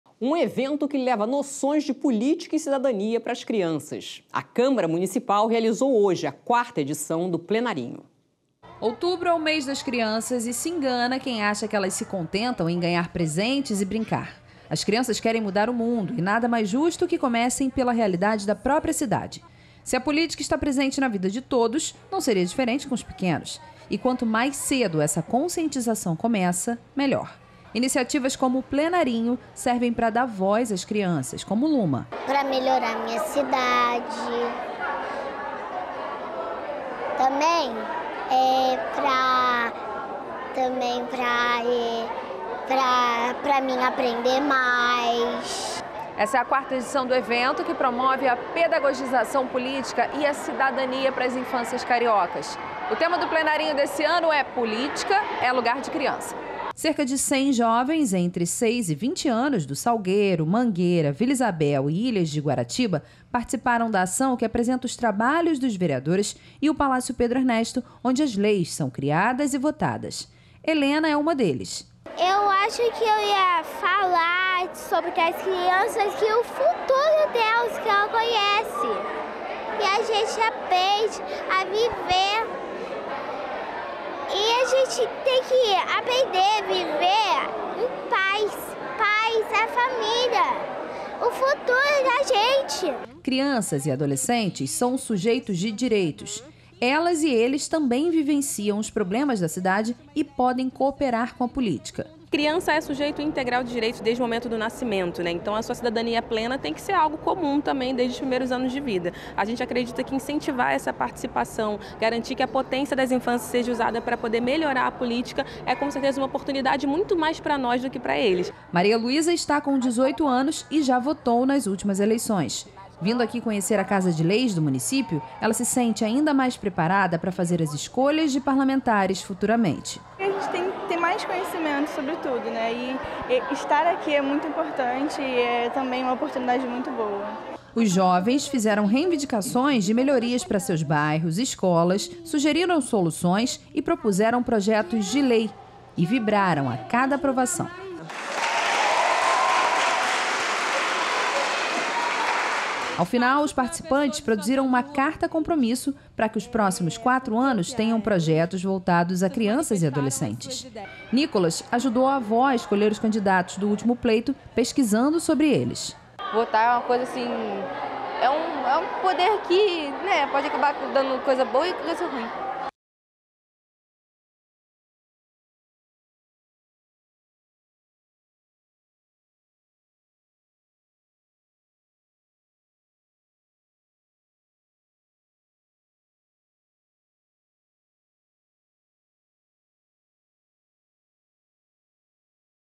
Crianças participam de plenário na Câmara do Rio
Câmara Rio Notícias (18/10/2024): Na quarta edição do Plenarinho, realizado pela Câmara do Rio, cem jovens entre seis e vinte anos de diversas comunidades, como Salgueiro, Mangueira, Vila Isabel e Ilha de Guaratiba, participaram de um evento especial em comemoração ao mês das crianças. O tema deste ano, "Política é lugar de criança", trouxe noções de cidadania e política para os pequenos, incentivando a participação ativa no futuro da cidade.

Durante o evento, as crianças visitaram o Palácio Pedro Ernesto, aprenderam sobre o trabalho dos vereadores e apresentaram soluções e reivindicações para melhorar os bairros onde vivem. Ao final, produziram uma carta compromisso com propostas para os próximos quatro anos. A Câmara Juvenil, projeto paralelo, ainda realizou uma aula virtual sobre o processo legislativo, permitindo que os jovens criassem propostas que podem virar leis municipais.